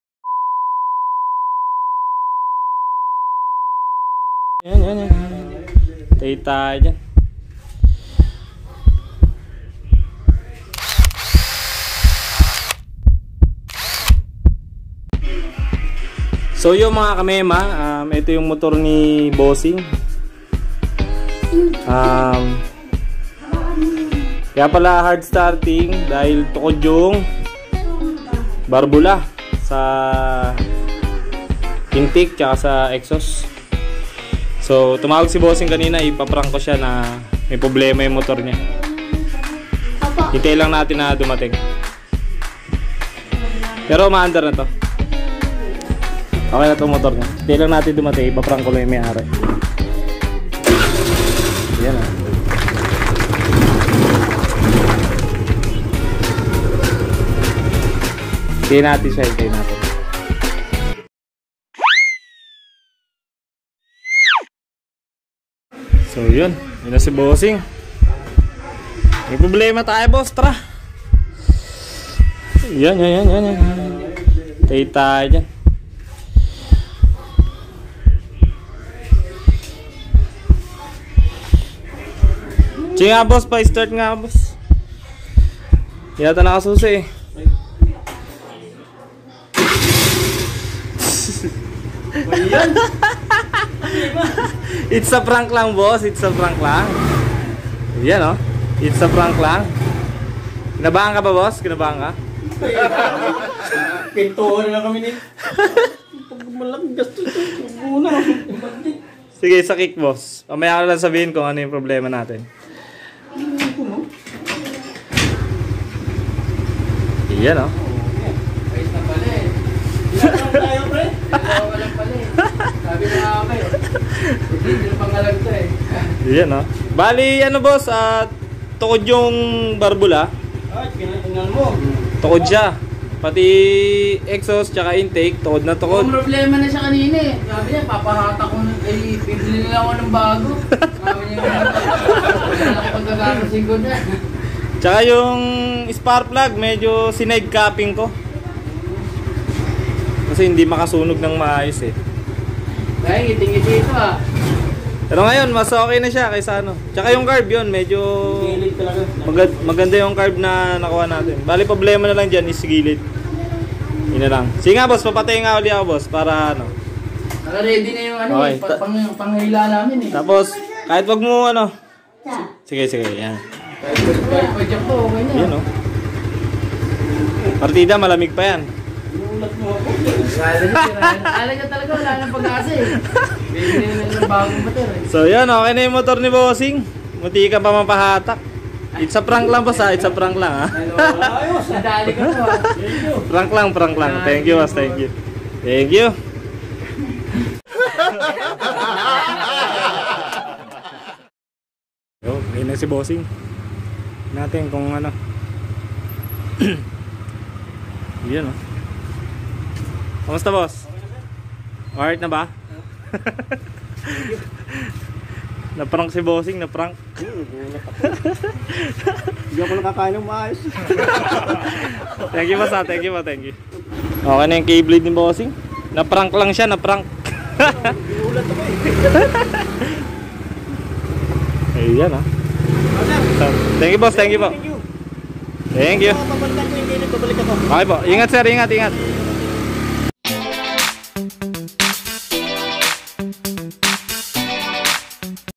KBLADE NI BOSSING NAKO MALALA ANG TAMA
PANO MAG LIPAT NG CHARGER ADAPTOR NG KBLADE
ITANANGGAL ANG KILL SWITCH SIDE STAND NI KBLADE